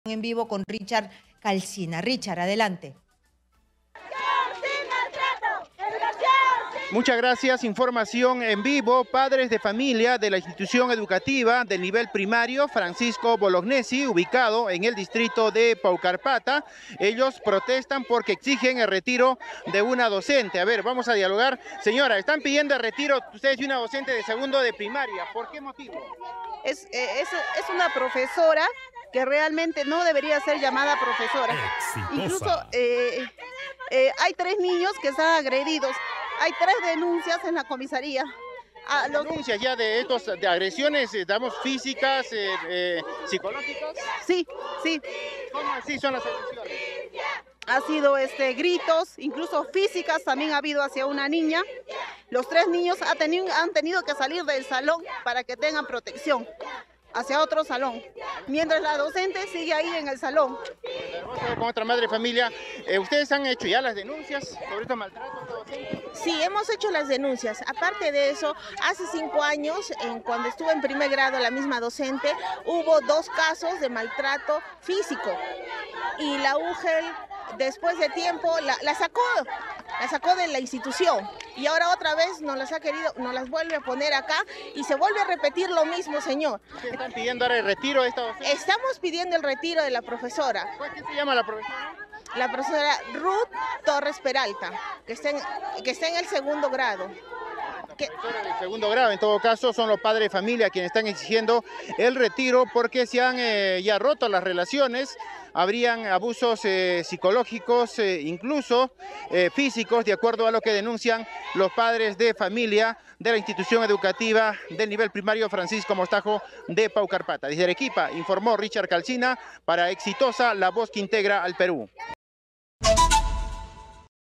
🔴🔵 Arequipa: Padres de colegio de Paucarpata exigen retiro de profesora por maltrato a escolares
Arequipa: Padres de familia de colegio de Paucarpata exigen retiro de profesora por maltrato a escolares. "Los tres niños han tenido que salir del salón para que tengan protección", indicó madre de familia.

Exitosa Perú
"Exitosa: La voz que integra al Perú"

Corporación Universal